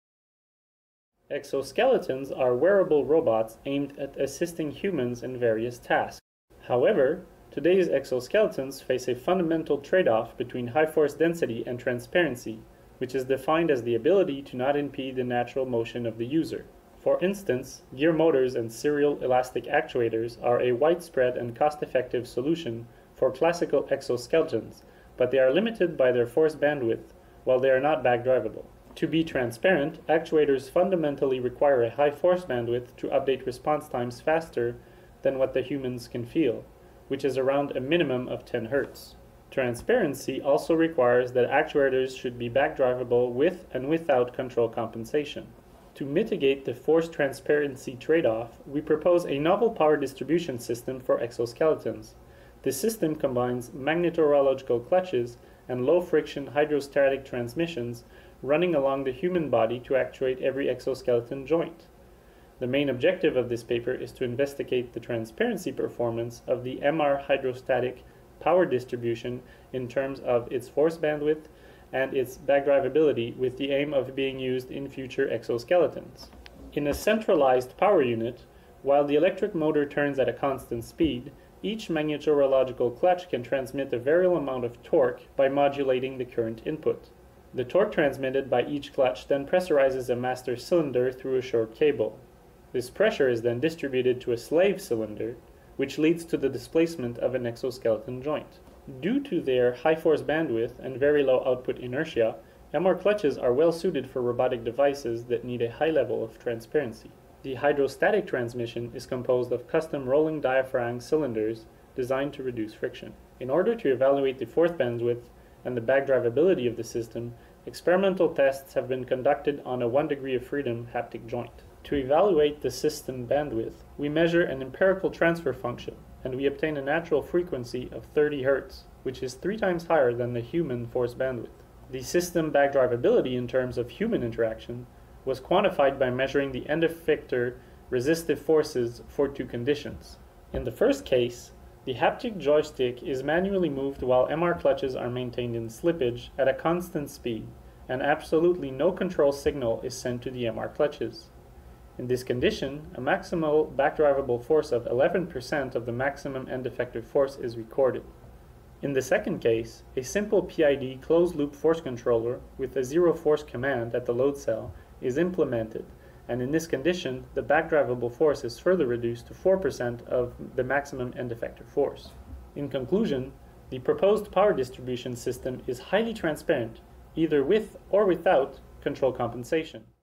A High-Bandwidth Back-Drivable Hydrostatic Power Distribution System for Exoskeletons Based on Magne
Interactive Session Wed PM Pod C.8
Authors: Veronneau, Catherine; Lucking Bigué, Jean-Philippe; Lussier Desbiens, Alexis; Plante, Jean-Sebastien
Title: A High-Bandwidth Back-Drivable Hydrostatic Power Distribution System for Exoskeletons Based on Magneto-Rheological Clutches

Abstract:
Exoskeletons are increasingly interesting for human assistance applications ranging from rehabilitation to force-enhancement. However, today's exoskeletons are relatively slow and lack the mechanical transparency required to complete several daily tasks, mainly due to their bulky and non-back-drivable actuation mechanisms. To improve upon conventional exoskeleton designs, this paper presents a novel power-distribution system that combines Magneto-Rheological (MR) clutches and low-friction hydrostatic transmissions using rolling diaphragms. In such a system, MR clutches are used to rapidly modulate the torque provided from a centralized power source and distribute it to each joint through a high-bandwidth, back-drivable and low-inertia transmission. The main objective of this paper is to investigate the transparency performance of the MR-Hydrostatic power distribution in terms of its force-bandwidth and back-drivability with the aim of being used in future exoskeletons. Experiments with a custom one degree-of-freedom haptic joint are supported by an analytical model that demonstrates the high bandwidth (greater than 40 Hz) and good back-drivability (2 to 11 % of peak force) of a MR-hydrostatic system.